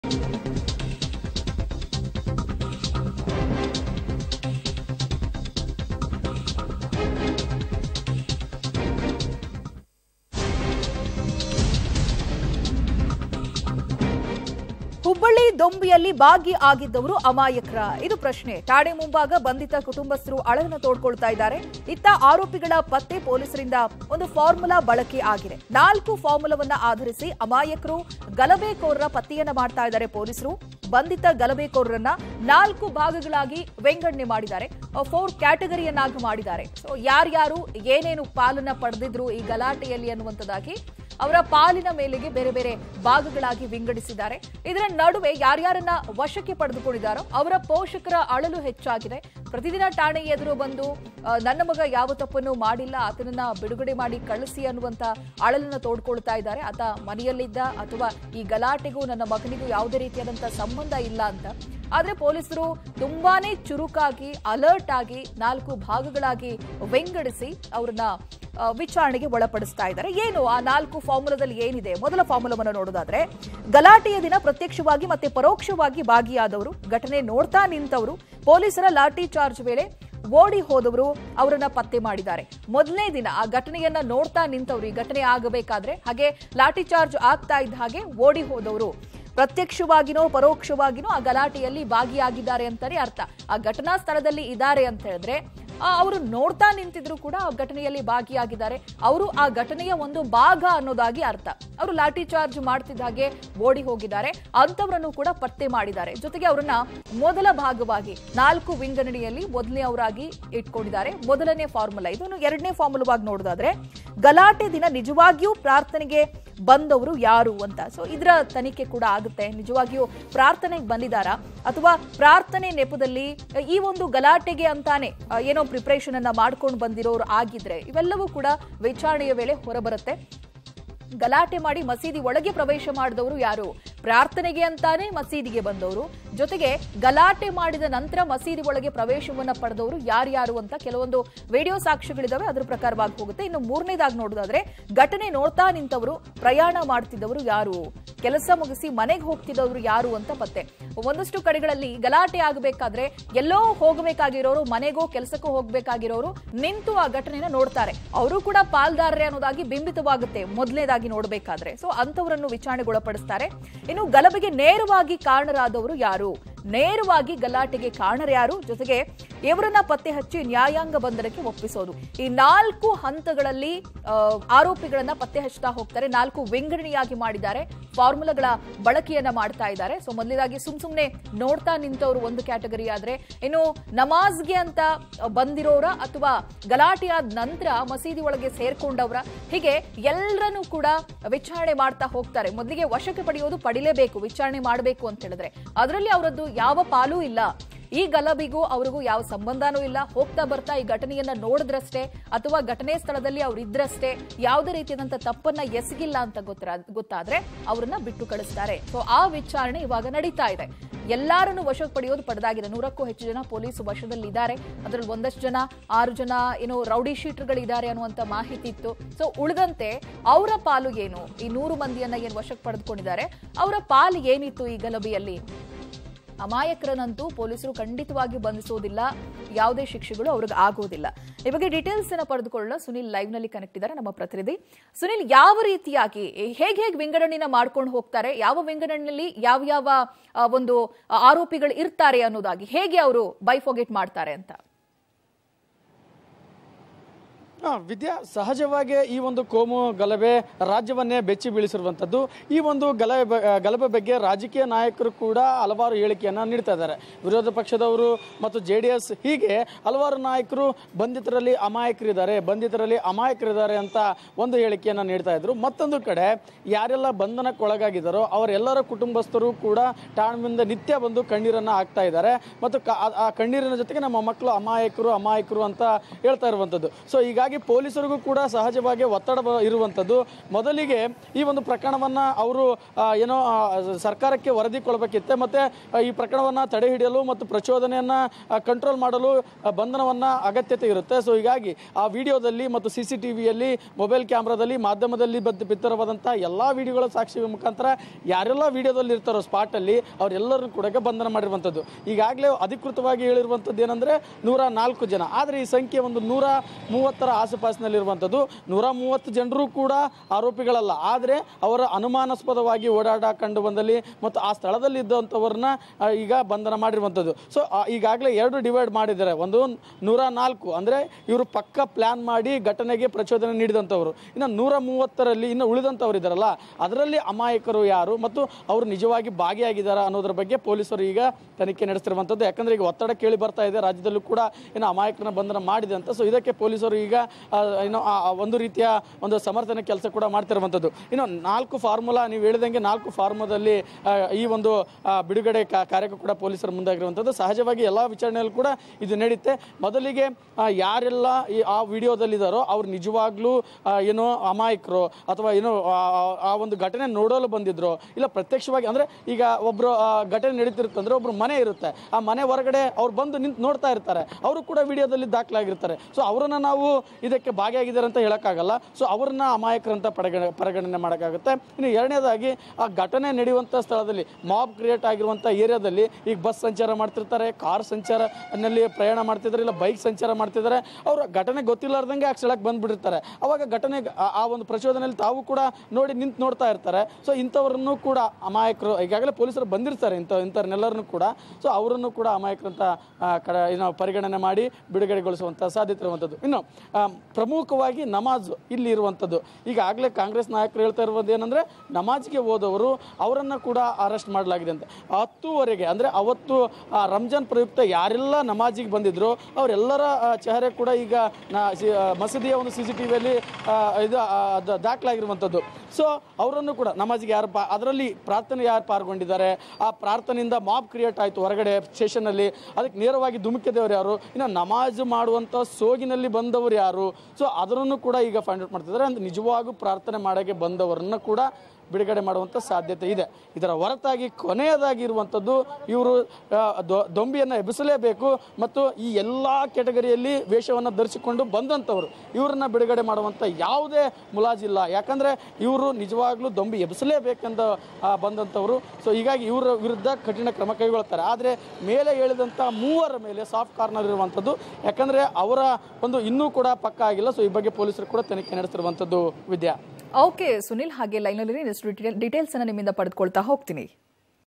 ಹುಬ್ಬಳ್ಳಿ ಗಲಭೆ ಆರೋಪಿಗಳ ಪತ್ತೆಗೆ ಪೊಲೀಸರಿಂದ ಫಾರ್ಮುಲಾ ಬಳಕೆ | Hubbali Riots Case
ಹುಬ್ಬಳ್ಳಿ ದೊಂಬಿಯಲ್ಲಿ ಭಾಗಿ ಆಗಿದ್ದವರು ಅಮಾಯಕರಾ..?
ಠಾಣೆ ಮುಂಭಾಗ ಬಂಧಿತ ಕುಟುಂಬಸ್ಥರ ಅಳಲು
ಇತ್ತ ಆರೋಪಿಗಳ ಪತ್ತೆ ಪೊಲೀಸರಿಂದ ಫಾರ್ಮುಲಾ ಬಳಕೆ
೪ ಫಾರ್ಮುಲಾ ಆಧರಿಸಿ ಅಮಾಯಕರು, ಗಲಭೆಕೋರರ ಪತ್ತೆ
ಬಂಧಿತ ಗಲಭೆಕೋರರನ್ನು ೪ ಭಾಗಗಳಾಗಿ ವಿಂಗಡಣೆ

ಗ್ರಾಫಿಕ್ಸ್
ಫಾರ್ಮುಲಾ-೧ ಘಟನೆಯಲ್ಲಿ ಭಾಗಿಯಾದವರು
* ಗಲಾಟೆಯ ದಿನ ಪ್ರತ್ಯಕ್ಷವಾಗಿ ಮತ್ತು ಪರೋಕ್ಷವಾಗಿ ಭಾಗಿಯಾದವರು
* ಘಟನೆ ನೋಡುತ್ತಾ ನಿಂತವರು, ಪೊಲೀಸರ ಲಾಠಿ ಚಾರ್ಜ್ ವೇಳೆ ಓಡಿಹೋದವರ ಪತ್ತೆ

ಫಾರ್ಮುಲಾ-೨ - ನಮಾಜ್‌ಗೆ ಹೋದವರು
* ಗಲಾಟೆ ದಿನ ನಿಜವಾಗಿಯೂ ಪ್ರಾರ್ಥನೆಗೆ ಬಂದವರು ಯಾರು..?
* ಗಲಾಟೆ ಮಾಡಿ ಮಸೀದಿಯೊಳಗೆ ಪ್ರವೇಶ ಮಾಡಿದವರು ಯಾರು ಎಂದು ಪತ್ತೆ

ಫಾರ್ಮುಲಾ-೩ - ಘಟನಾ ಸ್ಥಳದಲ್ಲಿ ಇದ್ದವರು
* ಘಟನೆ ನೋಡುತ್ತಾ ನಿಂತವರು, ಪ್ರಯಾಣ ಮಾಡುತ್ತಿದ್ದವರು ಯಾರು..?
* ಕೆಲಸ ಮುಗಿಸಿ ಮನೆಗೆ ಹೋಗುತ್ತಿದ್ದವರು ಯಾರು ಎಂದು ಪತ್ತೆ.

ಫಾರ್ಮುಲಾ-೪ - ಗಲಭೆಗೆ ಪ್ರಚೋದಿಸಿದವರು
* ಗಲಭೆಗೆ ನೇರವಾಗಿ ಕಾರಣವಾದವರು ಯಾರು..?
* ಇವರನ್ನು ಪತ್ತೆ ಹಚ್ಚಿ ನ್ಯಾಯಾಂಗ ಬಂಧನಕ್ಕೆ ಒಪ್ಪಿಸುವುದು